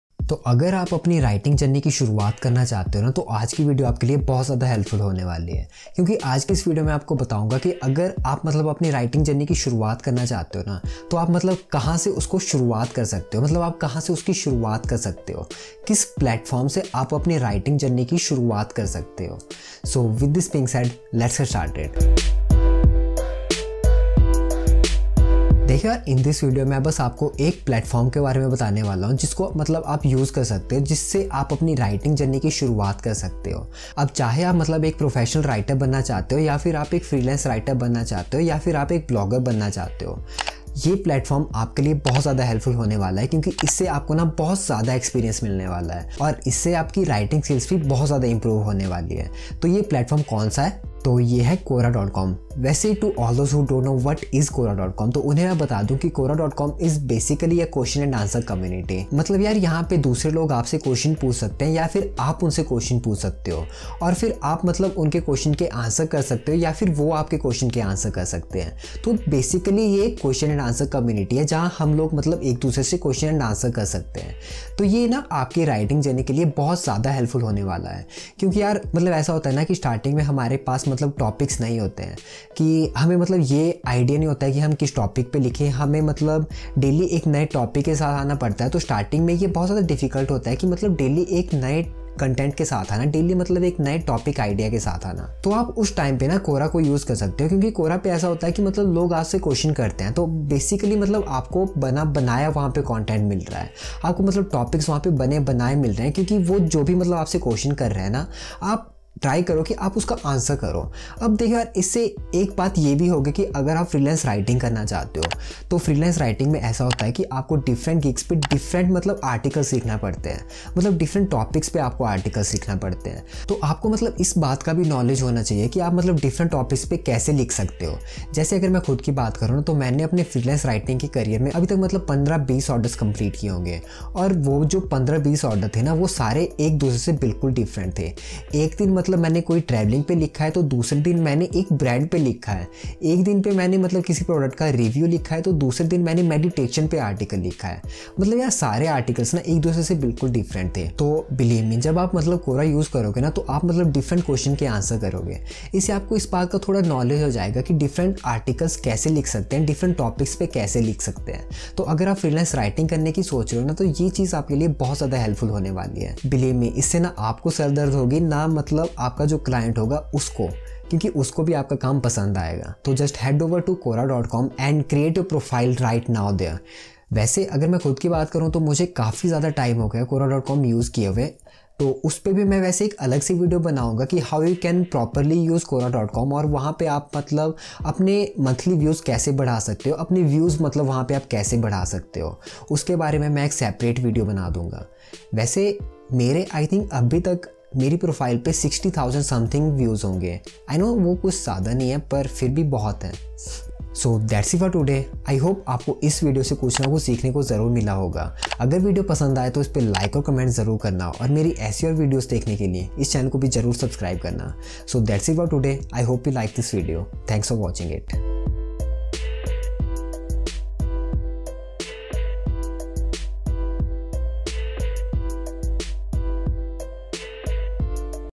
How to Get Started as a Writer | Where You Can Start Your Writing Journey [Hindi]
In this video, I will tell you where you can start your writing journey. How you can get started as a writer and where to start writing. I will share a platform in this video, where you can start your writing journey.

This platform will also be going to help you improve your writing skills and will be going to help you to become a better writer.

If you want to be a freelance writer, blogger, professional writer, or something like this, then this video is for you. I mean you just want to get started but don't know where to start then this video is going to help you in a really nice way.

I hope you will going to learn something from this video.

Watch the video to know more about it.